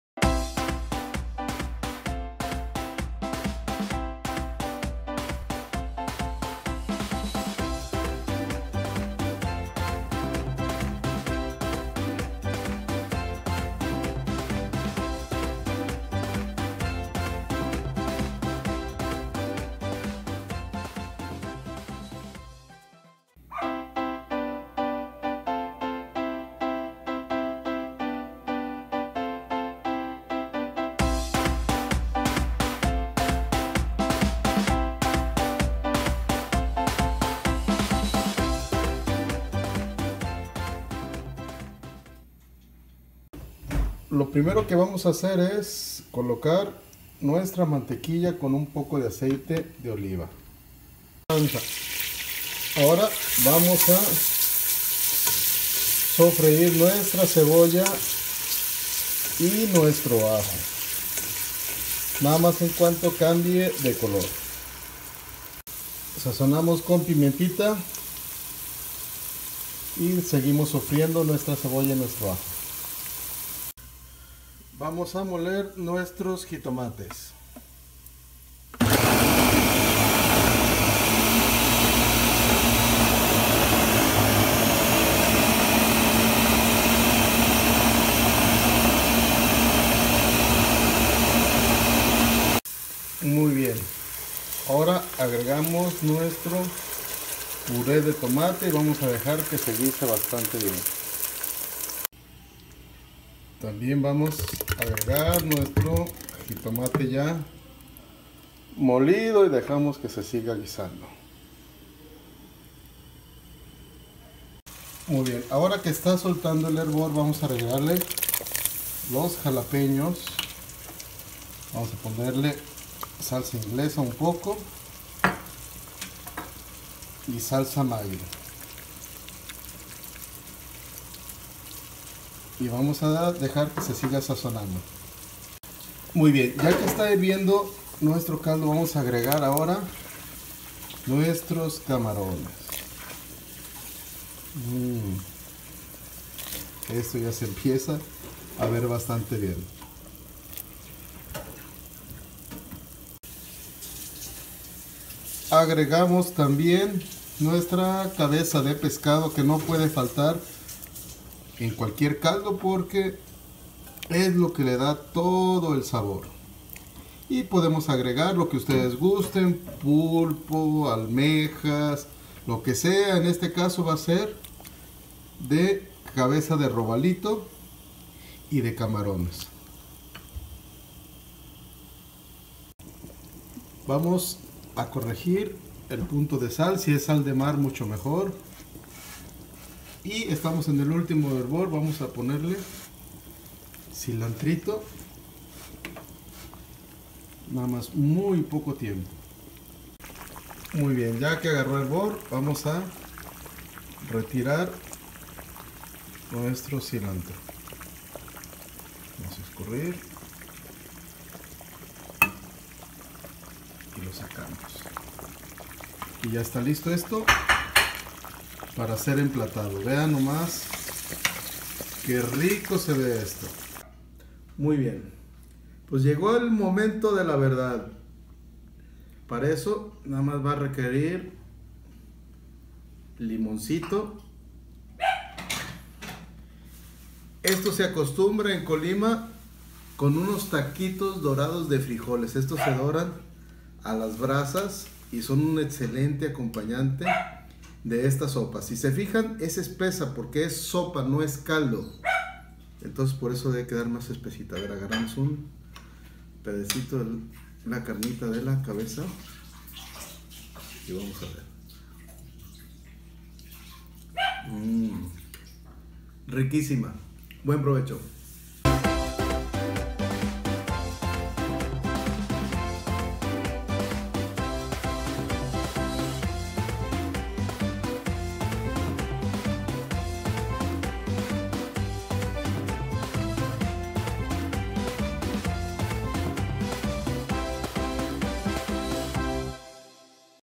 Sopa de Mariscos
INGREDIENTES:
1 kg de cabeza de pescado
1/2 kg de camarón
1 caja grande de puré de tomate
1 kg tomate maduro
1/2 cebolla
1/2 cabeza de ajo picada
1 barra mantequilla
Salsa Magui
Salsa Inglesa
1 lata de 200 gr de chiles jalapeños
1 manojo de cilantro